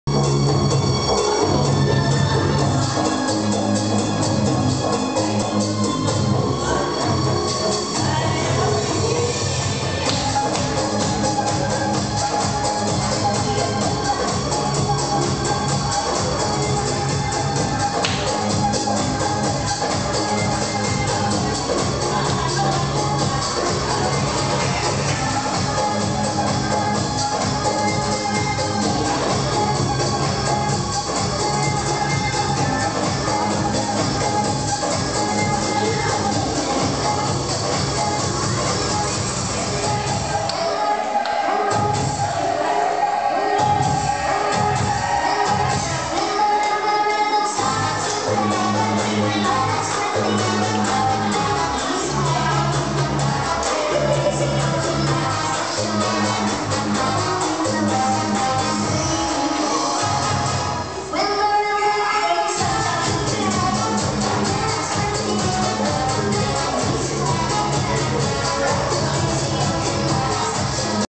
Kayamandi 4
Yet again, some more dancing from these kids! I couldnt get the camera down!